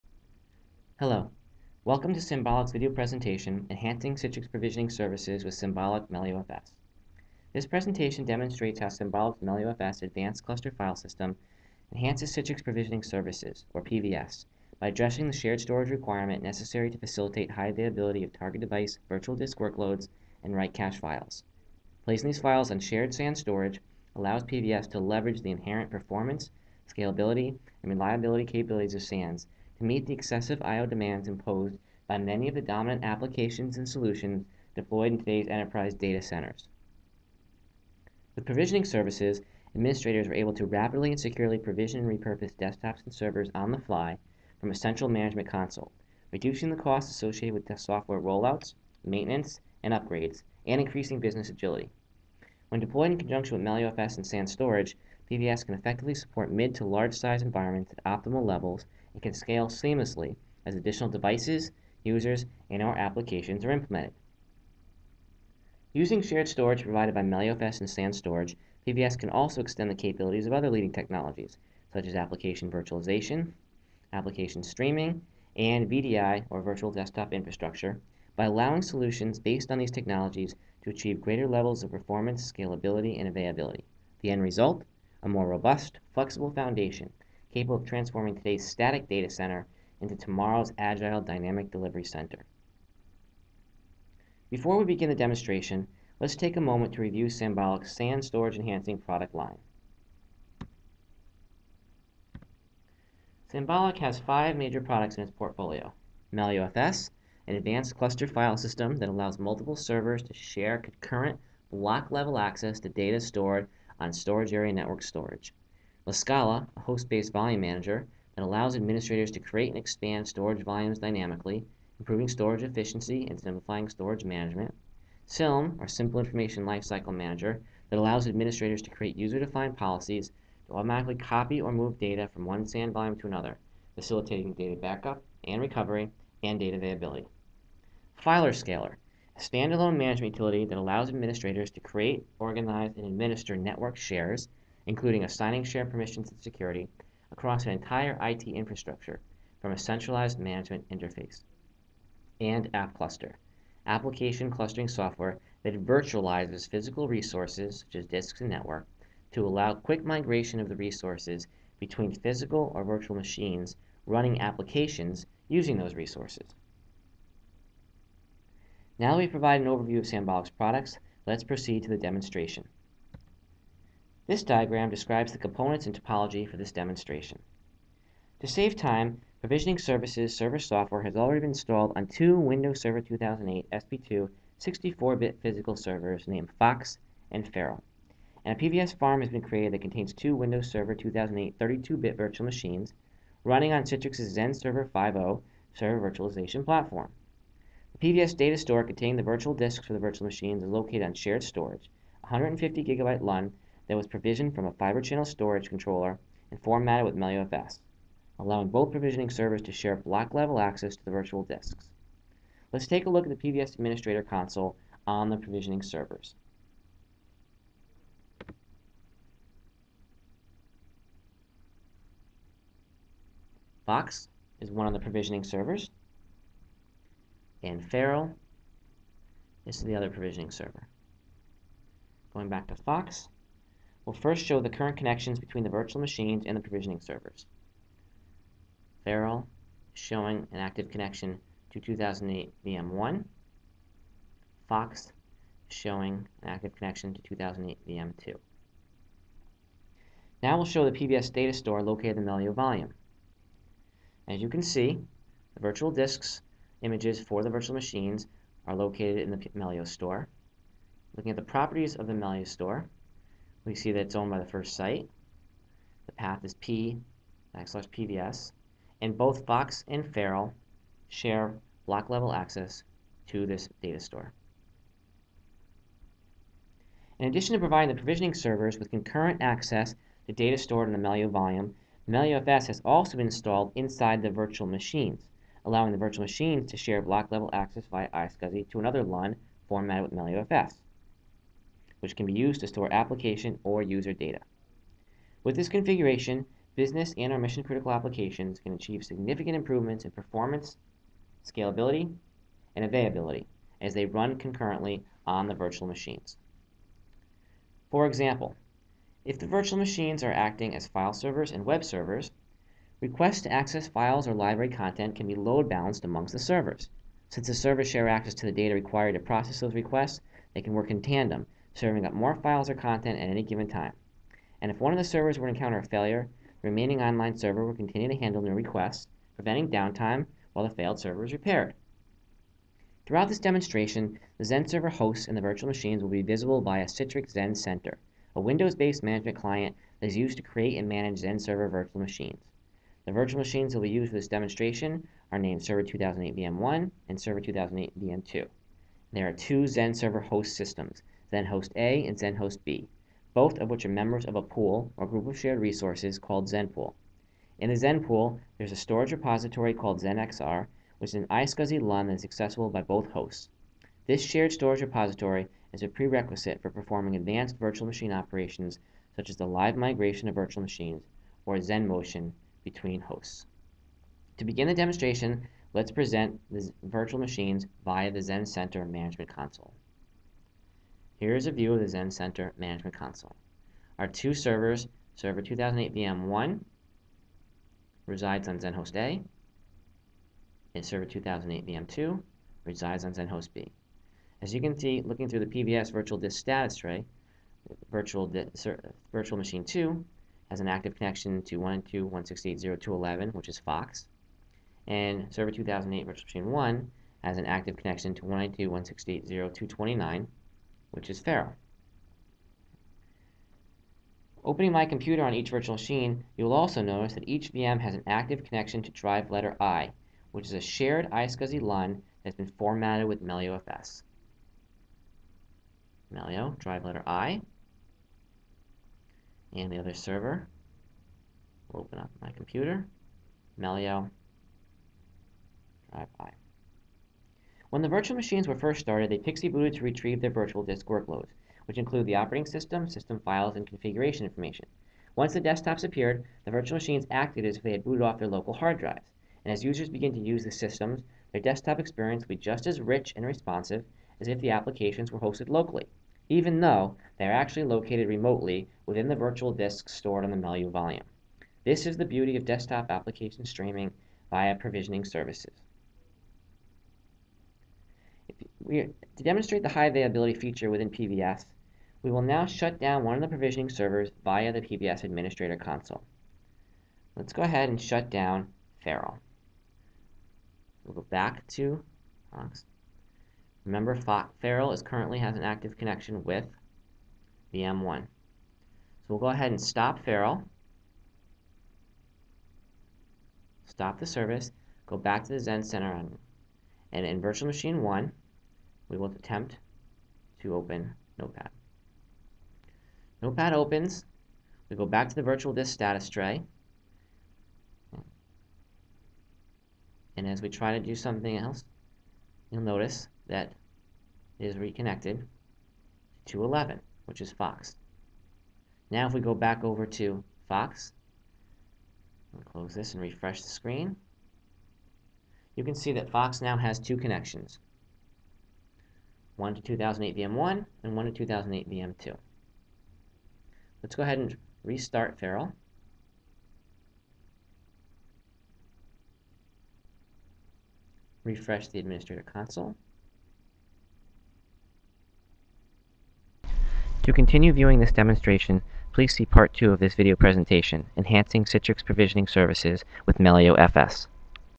Part 1: Enhancing Citrix Provisioning services with Sanbolic Melio FS
This two-part video presentation demonstrates how Sanbolic's Melio FS advanced cluster file system enhances Citrix Provisioning services by providing high-performance, scalable, flexible shared SAN storage to support highly available Provisioning services.